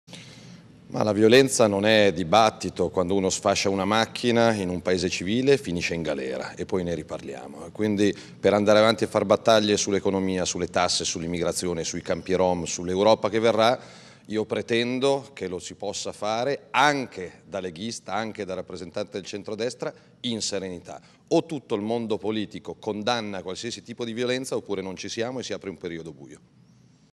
MATTEO SALVINI -Tutto il mondo politico condanni la violenza
«O tutti, senza se e senza ma, condanniamo e rifiutiamo ogni tipo di violenza, oppure io mi fermo»